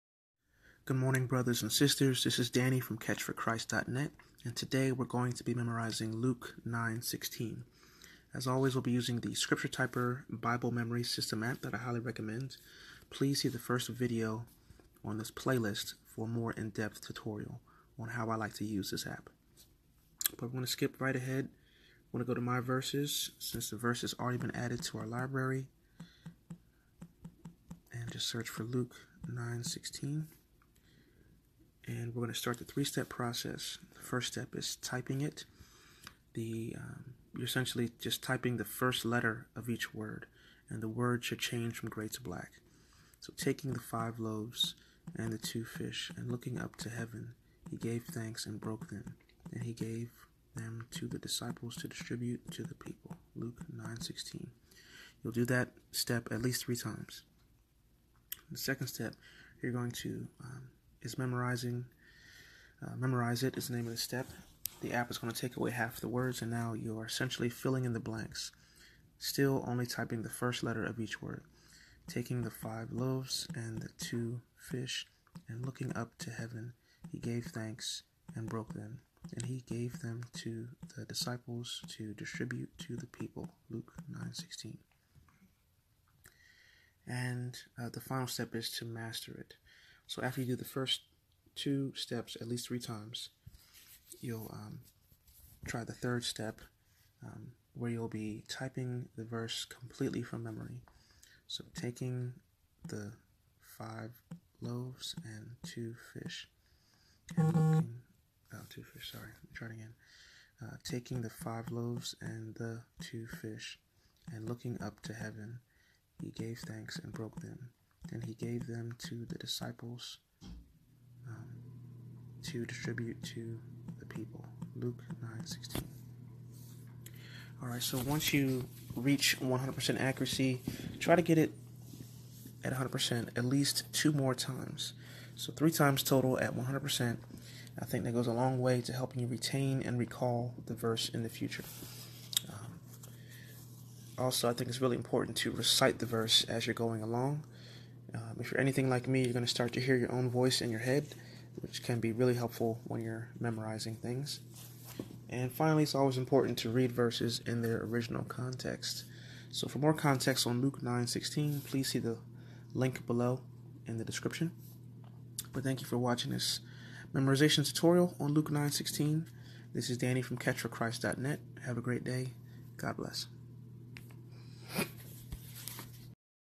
Luke 9:16 Memorization Tutorial (Video)
Memorize Luke 9:16 in less than five minutes after watching a brief video tutorial.